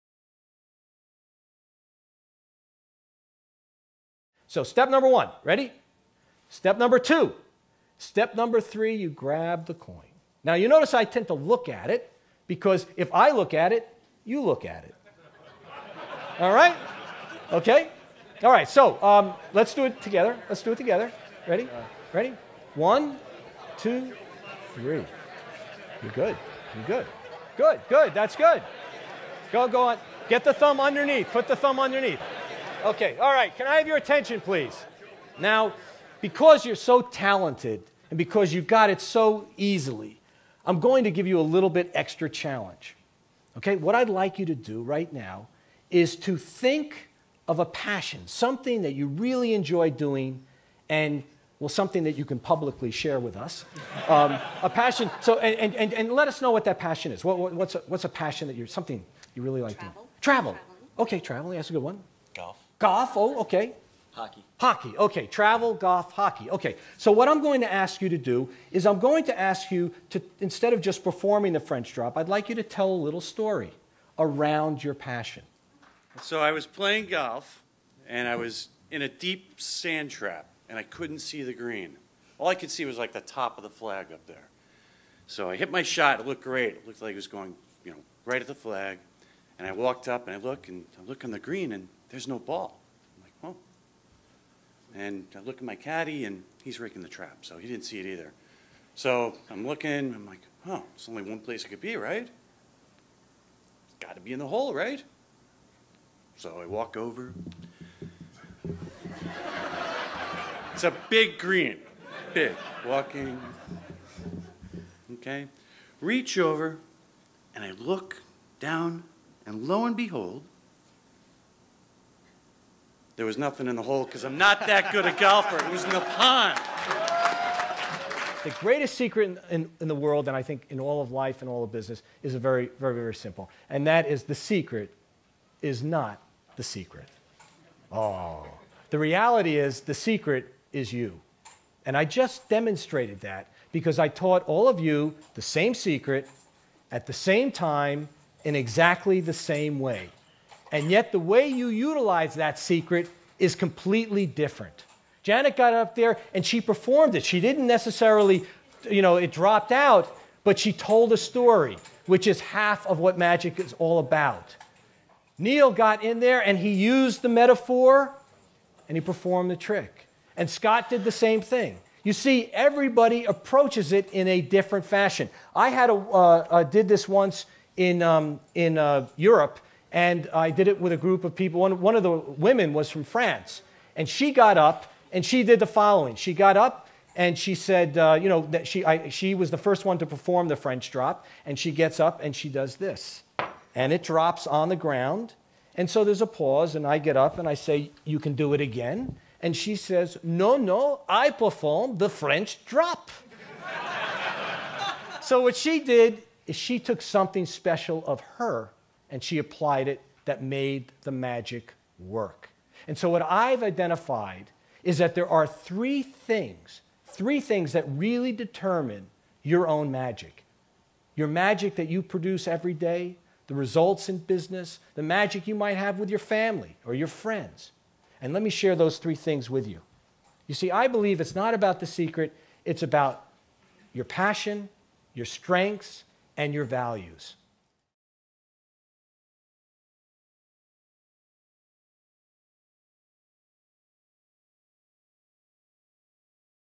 The World's Greatest Secret - Andy Cohen
Andy Cohen is founder of a global leadership and innovation advisory network. He has the privilege of working with the world's most respected brands while teaching at the world's most renowned universities.

Cohen is widely recognized as a leadership, innovation, and marketing guru, as well as a best-selling author. His client list includes American Express, J.P. Morgan Chase, HSBC, L'Oreal, Nestlé, Novartis, Standard Bank, and Time Warner.